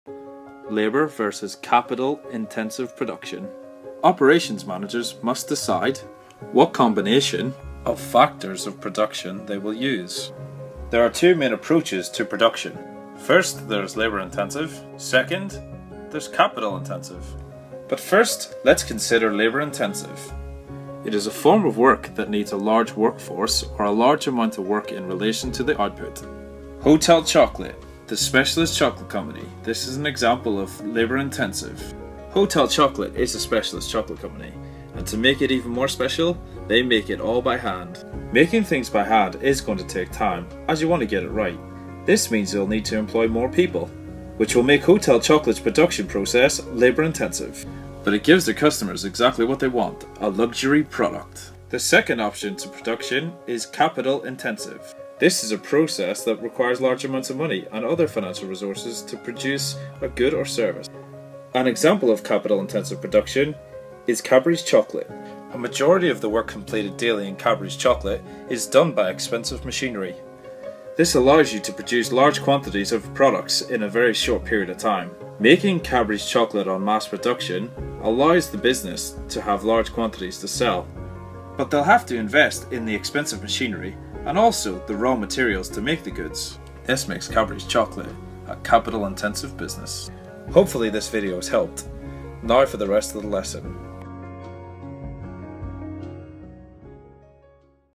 Labour vs. Capital intensive production
Explaining the difference with examples from the chocolate industry